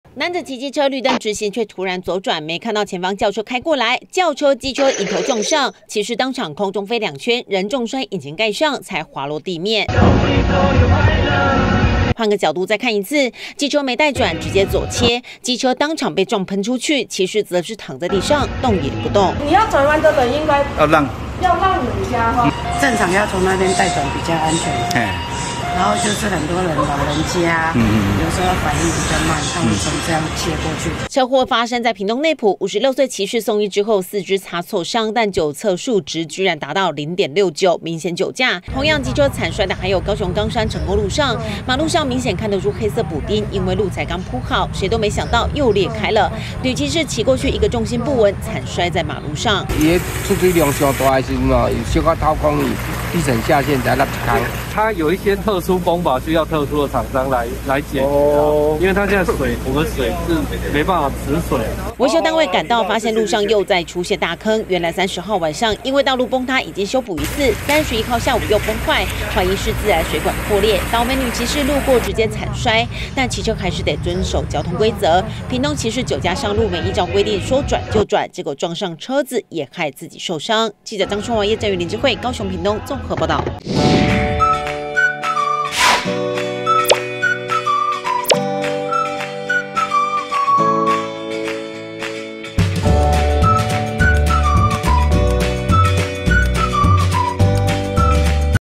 【每日必看】男酒駕"沒待轉"迎面猛撞汽車 拋空中飛2圈重摔 20240602
屏東內埔發生驚悚車禍，一名騎士沒待轉，直接往左撞上直行轎車，騎士人當場噴飛 前空翻2圈，重摔落地，高雄則是又出現天坑，補完一次，隔天又破洞，女騎士路過慘摔 撞傷頭，施工單位發現是水管漏水，緊急搶修。